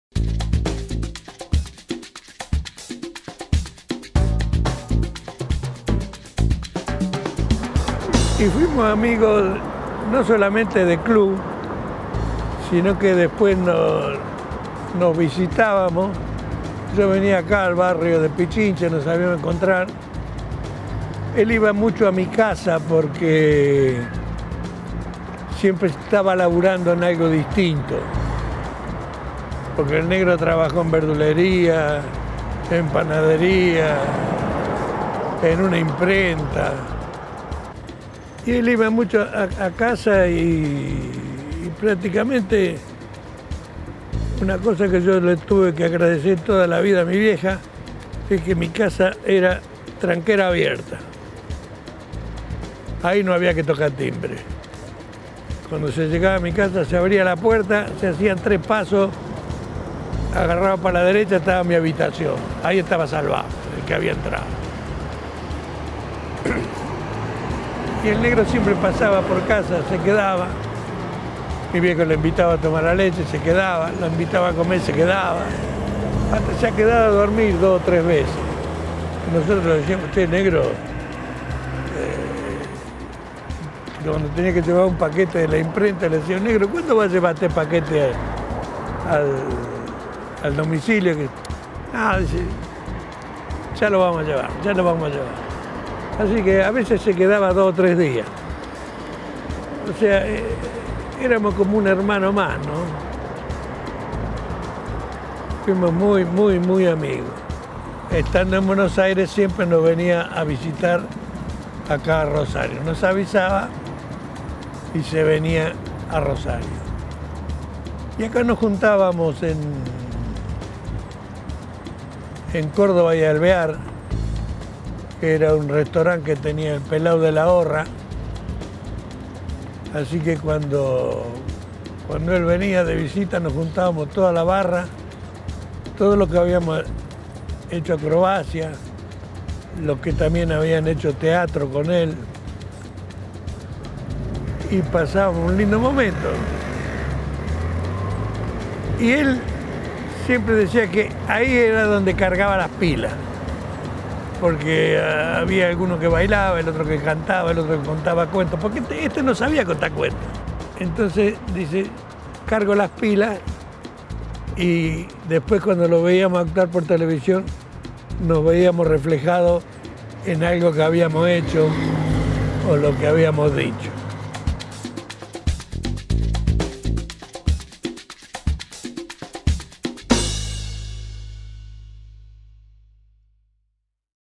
Estatua de Olmedo (Circuito Alberto “Negro” Olmedo)
“Chiquito” Reyes habla sobre la juventud de Alberto Olmedo en Rosario y su regreso frecuente a la ciudad, siendo ya un humorista reconocido, para cargar las pilas y buscar inspiración.